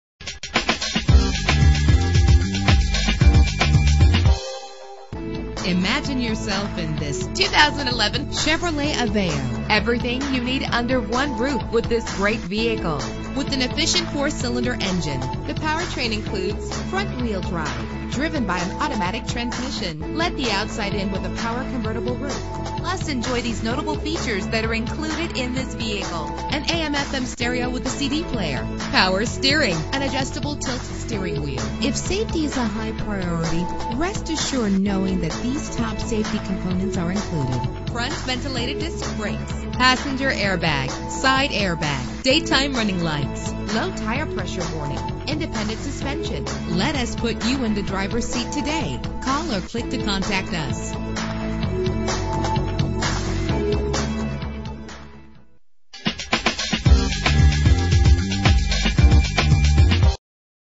2011 Chevrolet Aveo - Orlando FL
Year: 2011
Make: Chevrolet
Model: Aveo
Trim: LT
Engine: 1.6 liter inline 4 cylinder 16 valve
Transmission: Automatic
Color: White
Mileage: 36111
Address:
9001 E Colonial Dr